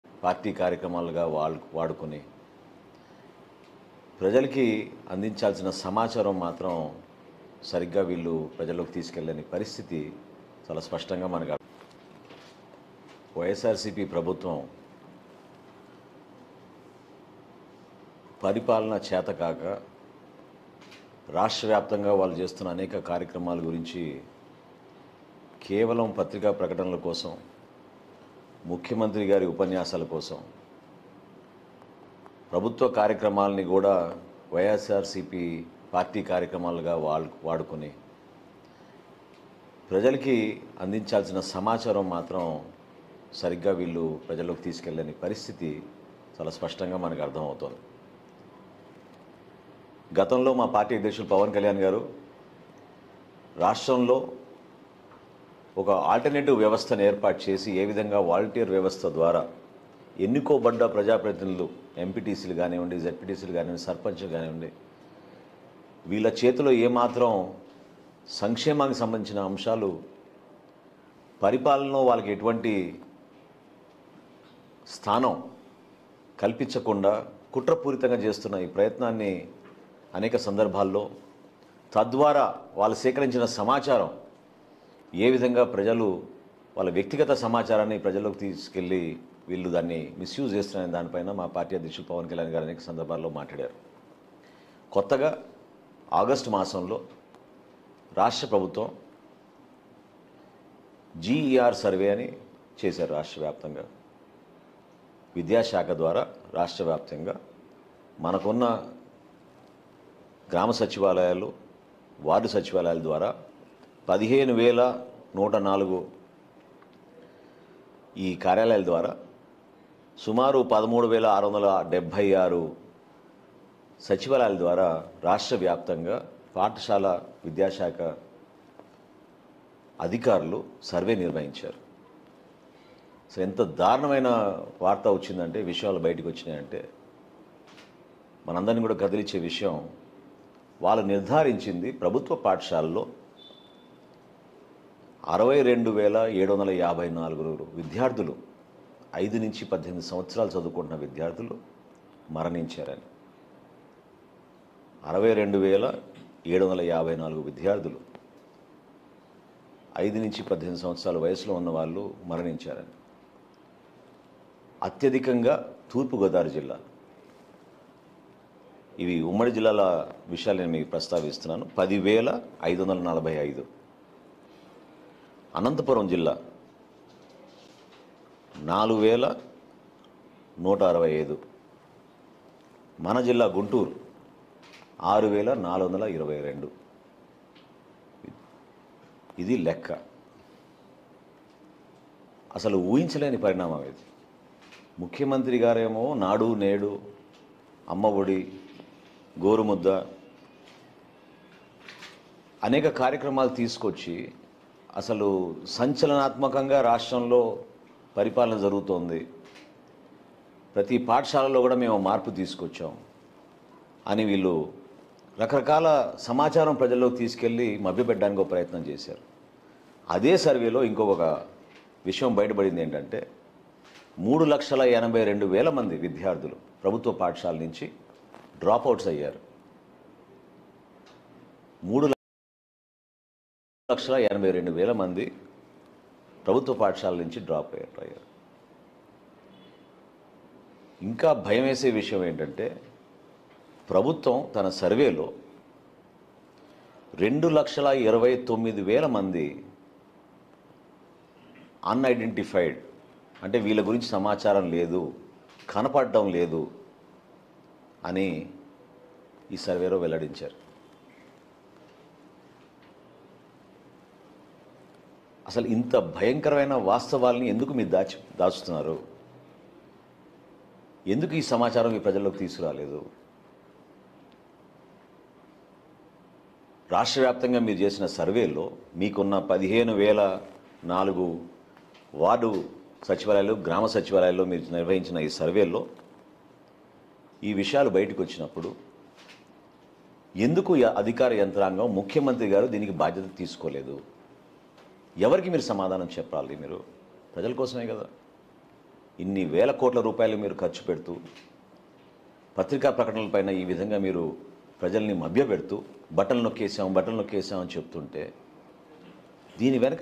ఎందుకు దాస్తున్నావ్ జగన్.. - Nadendla Manohar STRAIGHT Question To YS Jagan || Pawan Kalyan | CVR
వారాహి ఫేస్ 4 పై నాదెండ్ల ప్రెస్ మీట్🔴LIVE | Janasena Nadendla Manohar Press Meet | CVR